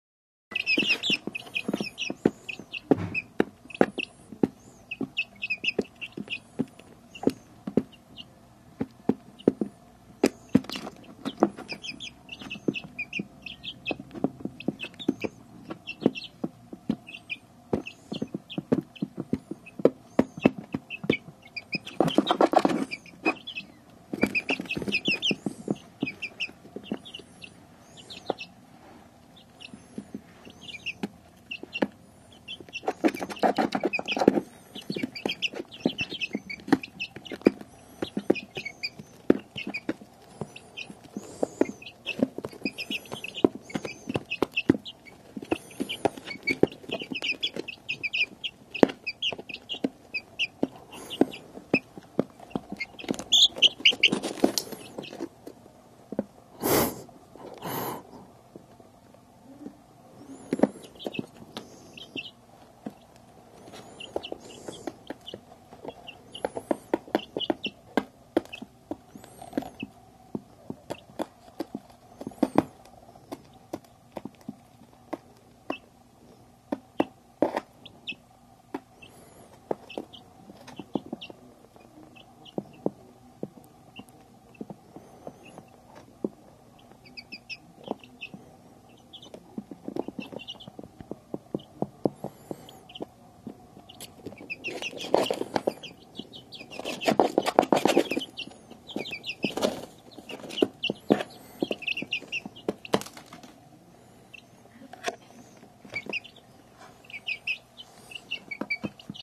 Got 3 Chicks!
My new chickens greet me. :) One of them stopped, and took a really long look at me...